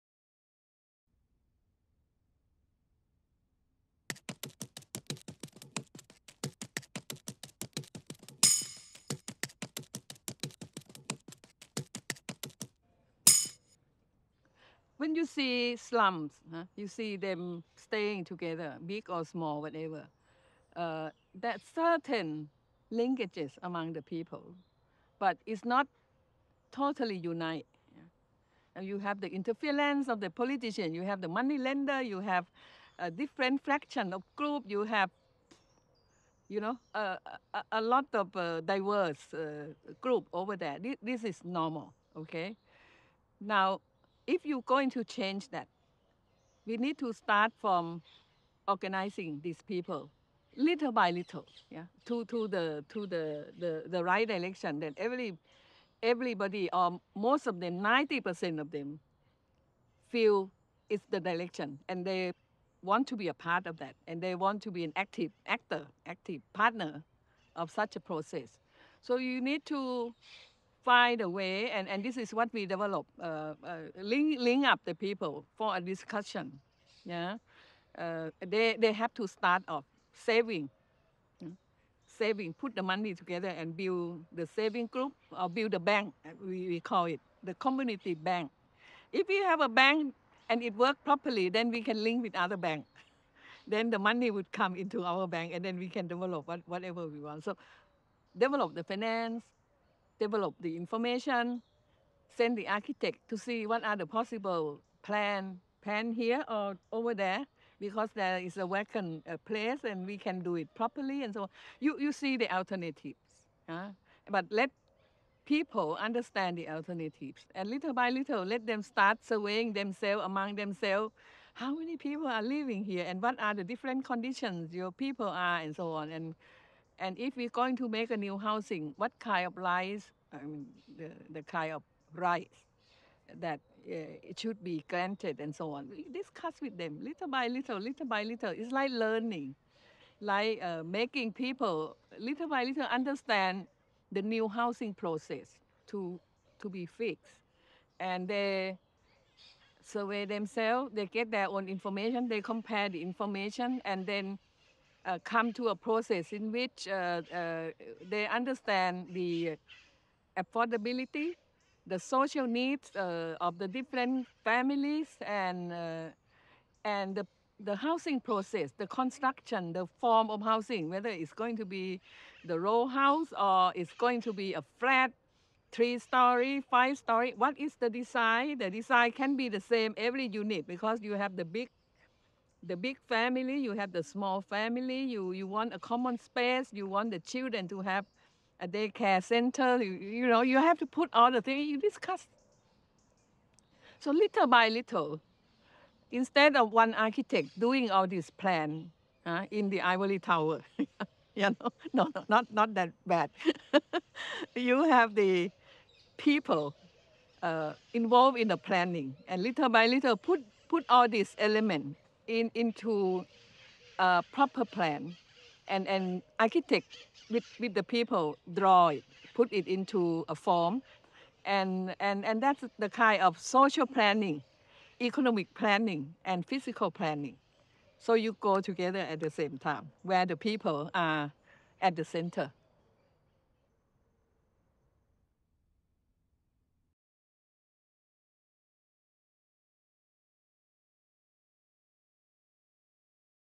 Collective Planning: Putting People at the Center
This video is part of the Accelerating City Equity (ACE ) project created by the International Society of Urban Health (ISUH). See more stories at ISUH.org/ACE.

Boonyabancha describes the process for engaging stakeholders developed by her team in Thailand, and how collaboration between local government, residents and other community stakeholders has lead to more successful, equitable and sustainable urban development in cities throughout Thailand.

Somsook Boonyabancha, is Chairperson, Asian Coalition for Housing Rights (ACHR), a regional network of grassroots community organizations, NGO’s and professionals actively involved with urban poor development processes in Asian cities; former Director of the Community Organization Development Institute (CODI); Chairperson, of the Baan Mankong Program Committee. Boonyabancha has been working and facilitating sustainable, equitable collective housing development and slum upgrading in Thailand and other Asian countries for 30 years – specializing in community-led initiatives. As director of CODI, she fostered a “city-wide” approach to community upgrading that has been replicated in cities across Thailand.  She is based in Bangkok, Thailand.

Masterclass 1: Engaging Communities to Drive City-Wide Change

We encourage sharing of this material for non-commerical use. Proper attribution required. No derivatives or alterations permitted.